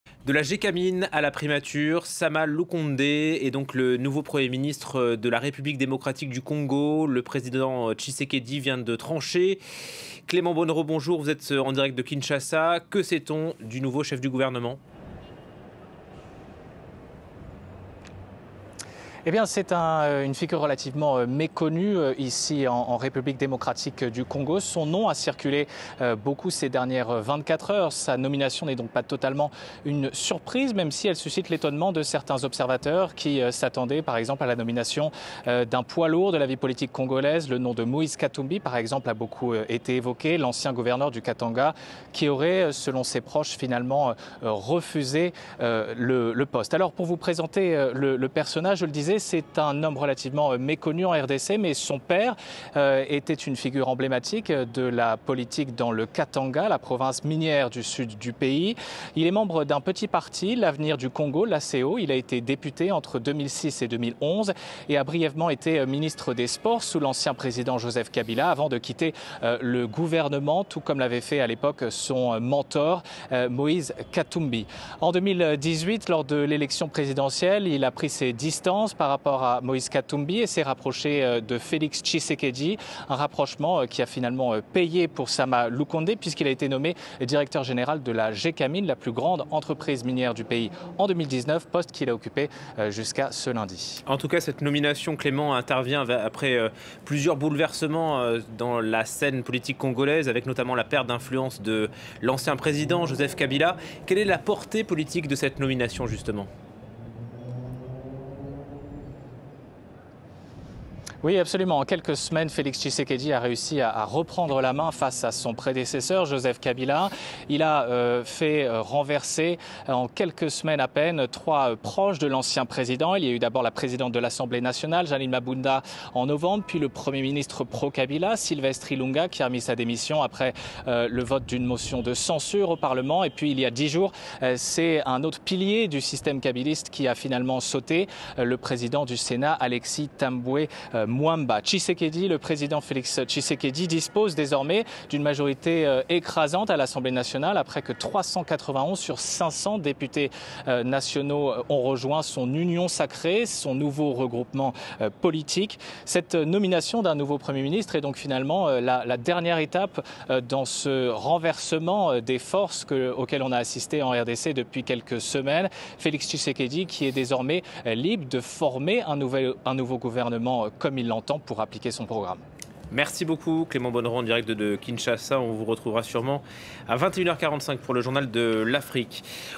Nouveau gouvernement en RDC : Sama Lukonde nommé Premier ministre
Jean-Michel Sama Lukonde Kyenge a été nommé Premier ministre du gouvernement d’Union sacrée par le président Tshisekedi. L’annonce a été faite le 15 février dans l’après-midi.